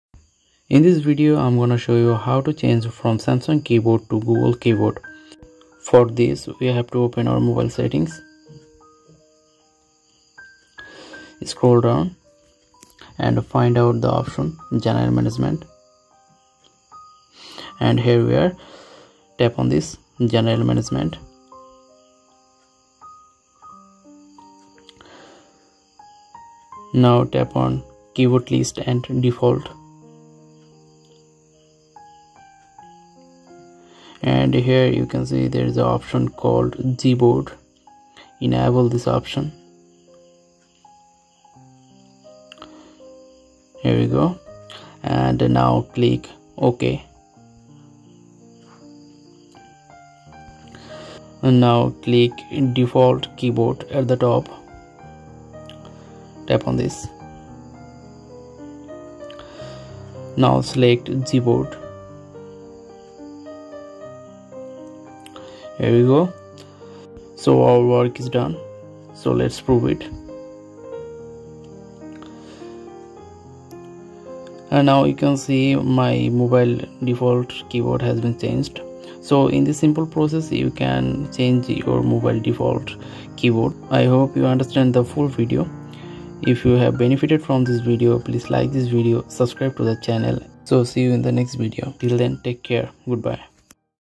How To Change Keyboard On Samsung Phone || Default Keyboard To Gboard || Google Keyboard
Today we will learn How To Change Keyboard On Samsung Phone.
Default Keyboard To Gboard.
Google Keyboard.

Video Step:
Open your mobile settings then find out general management,tap on this and the rest of process is in this video.

Copyrights: Tech Process